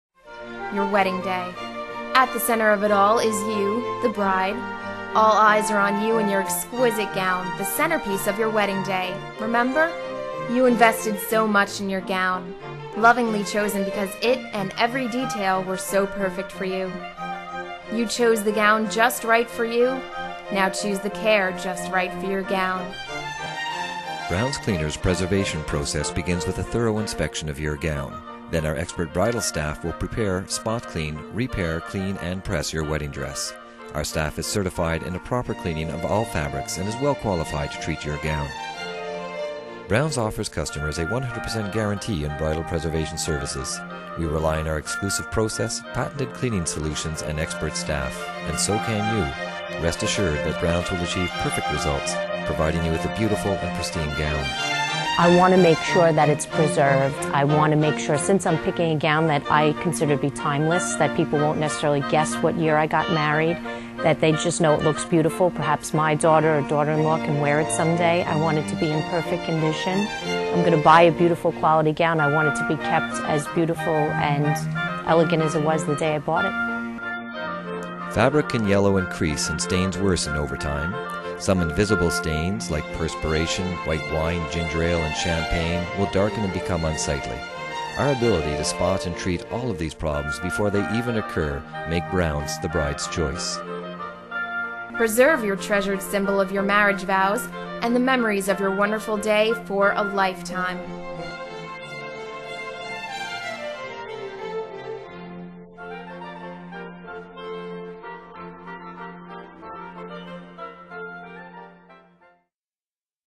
Cleaning & Preserving your Gown
A drop of champagne can ruin your wedding gown...
A trip to Browns Cleaners can save it!

Many brides find, to their dismay, that stains left untreated can damage their gown irreparably. It's almost certain that your gown will be stained over the course of your wedding day — whether the stain is immediately visible or not.

See how Browns Cleaners can help your wedding gown remain beautiful!